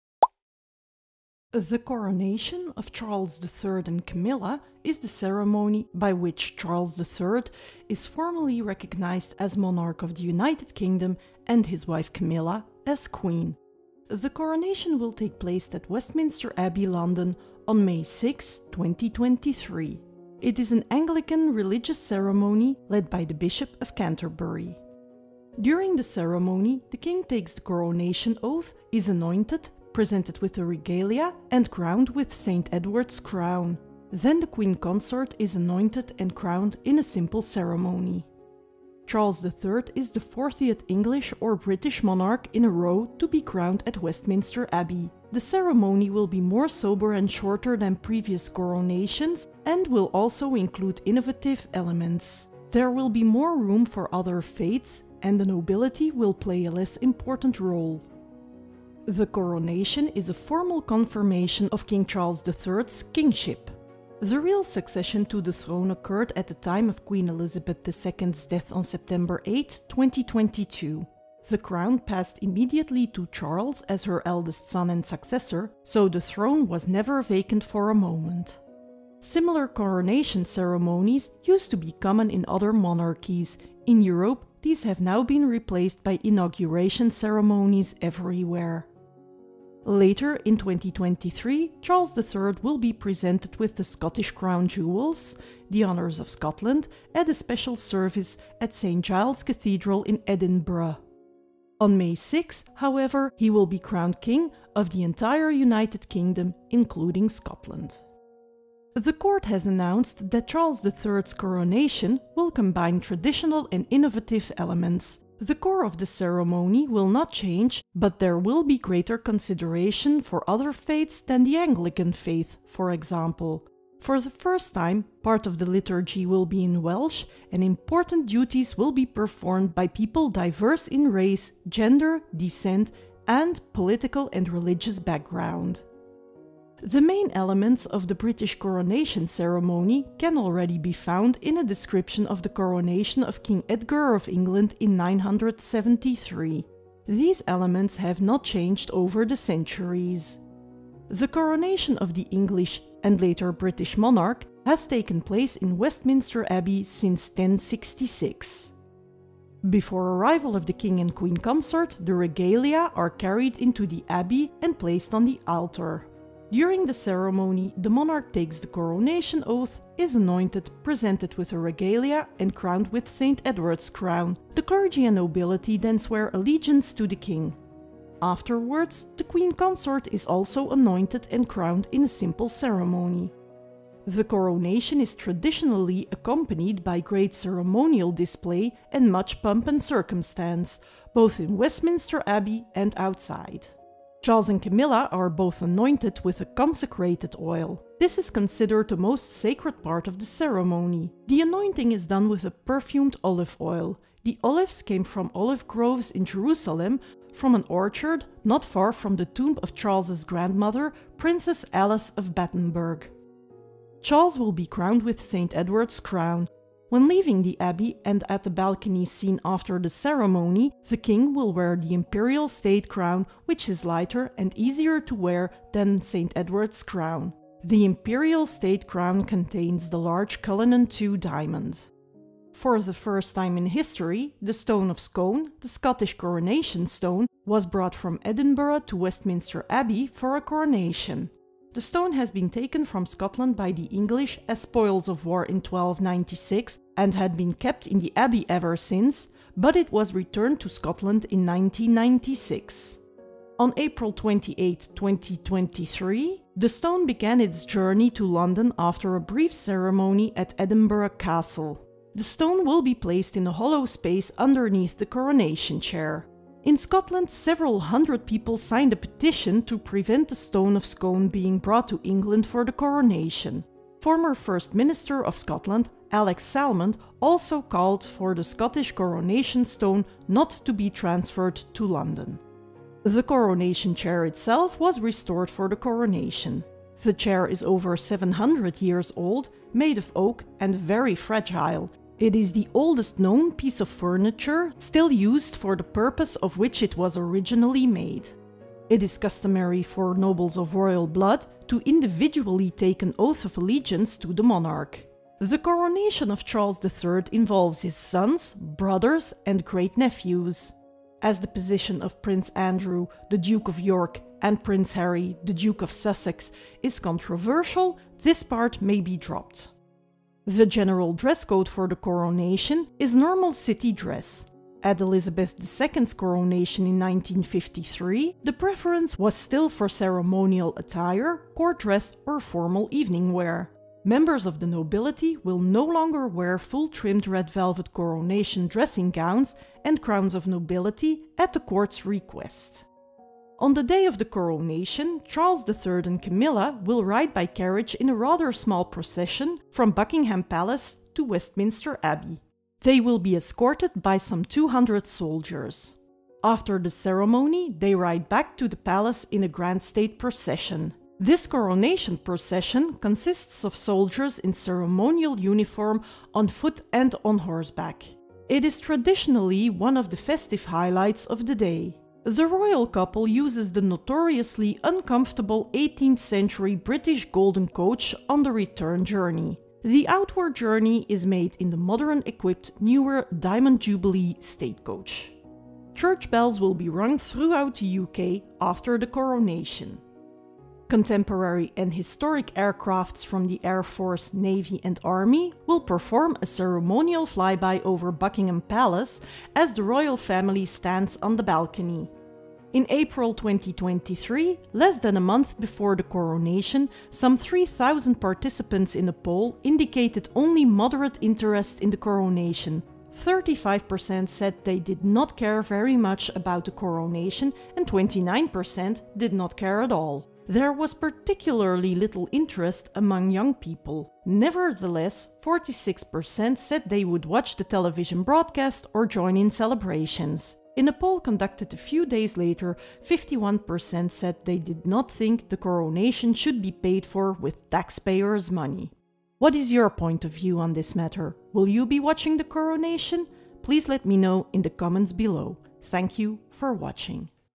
The Coronation Of Charles III - Short Overview
The Coronation Of Charles III - Short Overview: The coronation of Charles III and Camilla is the ceremony by which Charles III is formally recognised as monarch of the United Kingdom, and his wife Camilla as queen. The coronation will take place at Westminster Abbey, London on 6 May 2023. It is an Anglican religious ceremony led by the Archbishop of Canterbury.
During the ceremony, the king takes the coronation oath, is anointed, presented with the regalia (coronation jewels) and crowned with St Edward's crown. Then the queen consort is anointed and crowned in a simple ceremony.
Charles III is the fortieth English or British monarch in a row to be crowned at Westminster Abbey. The ceremony will be more sober and shorter than previous coronations, and will also include innovative elements. There will be more room for other faiths and the nobility will play a less important role.